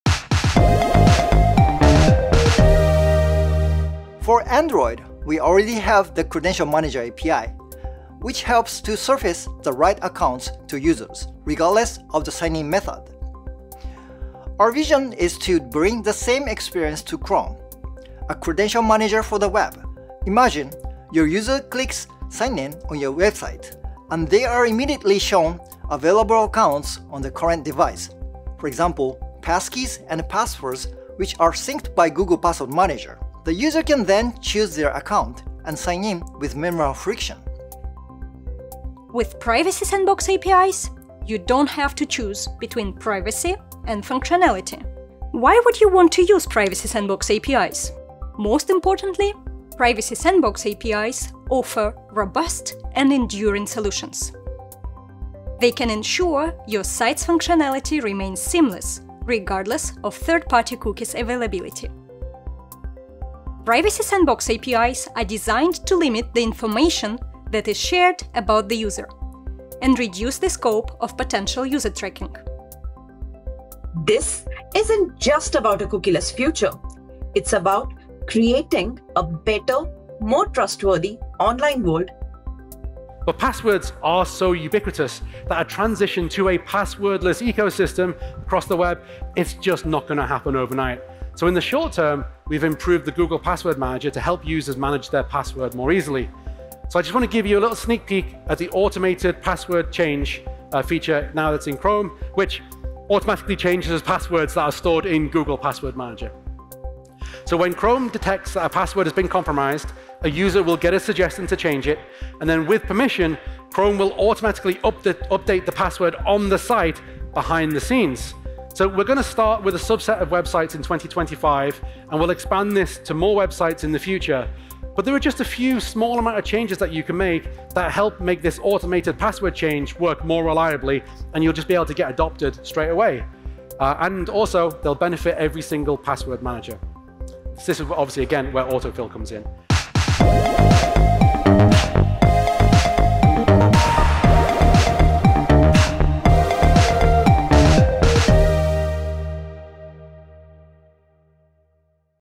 Top Identity and Privacy updates from Google I/O 2025
Learn about our latest identity and privacy announcements, from privacy and functionality with privacy sandbox to the improved Google Password Manager.

Chapters:
0:00 - Seamless sign-in: passkeys and more
0:39 - Privacy and functionality with Privacy Sandbox
1:14 - Control cookies with DevTools Privacy Panel
1:26 - Automated password change in Chrome

Products Mentioned: Chrome,  Privacy Sandbox for Chrome